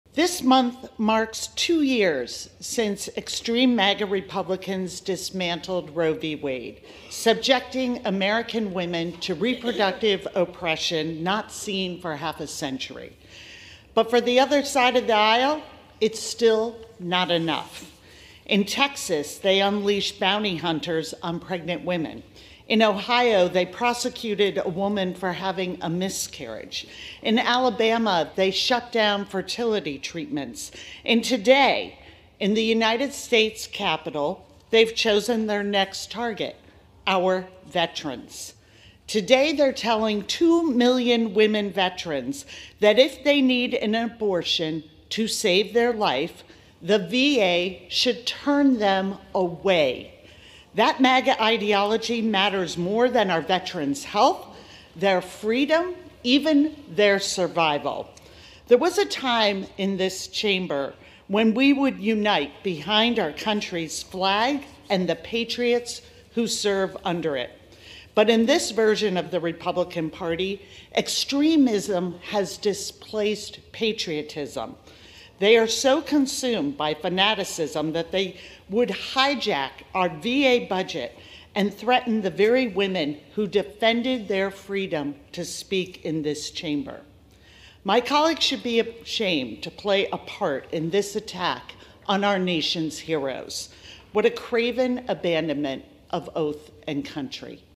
Whip Clark: “In This Version of the Republican Party, Extremism Has Displaced Patriotism.”
Democratic Whip Katherine Clark (MA-5) took to the House Floor to denounce Republicans' attempt to hijack the VA’s budget and ban veterans from accessing reproductive health care.